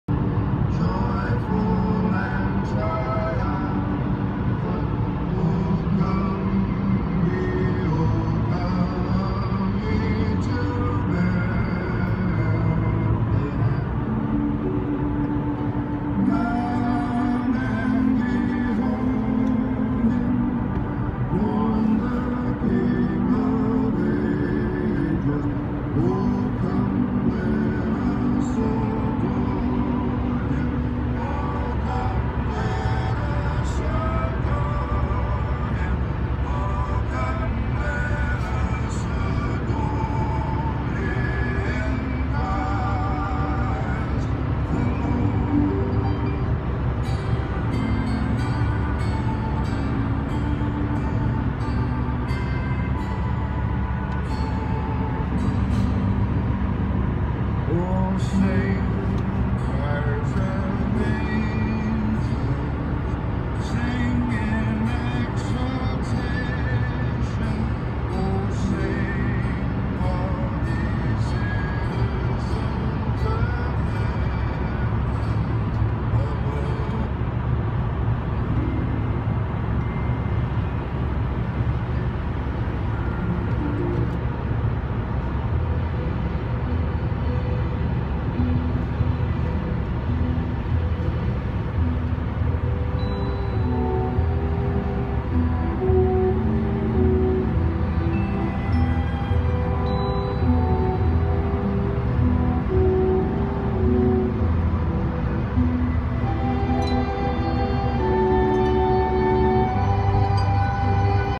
Driving view Montreal to USA (Vermont)..by Sammy Djamiraga.
I was driving a car from Canada-Montreal to USA via State of Vermon,after spent holiday thanksgiving at Quebec (Montreal).

This is mf first time driving a car to Canada. ( Nov 30,2015).
By Samuel "Sammy" Djamiraga.